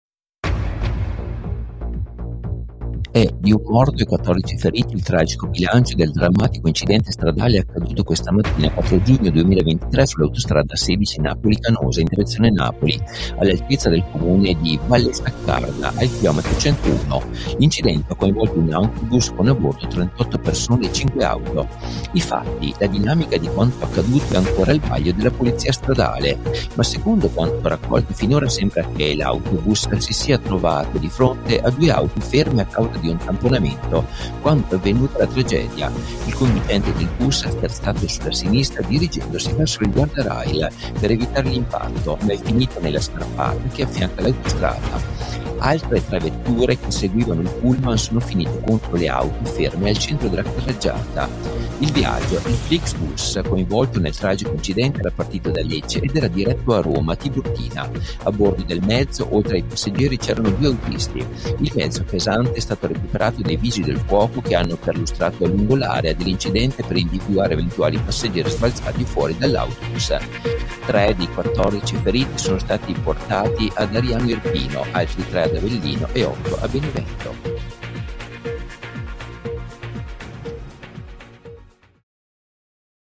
Tragedia sulla A16: morto un uomo e 14 feriti nello scontro tra un Flixbus e cinque auto
È di un morto e 14 feriti il tragico bilancio del drammatico incidente stradale accaduto questa mattina (4 giugno 2023) sull'autostrada A16 Napoli-Canosa, in direzione Napoli, all'altezza del comune di Vallesaccarda al km 101. L'incidente ha coinvolto un autobus con a bordo 38 persone e 5 auto.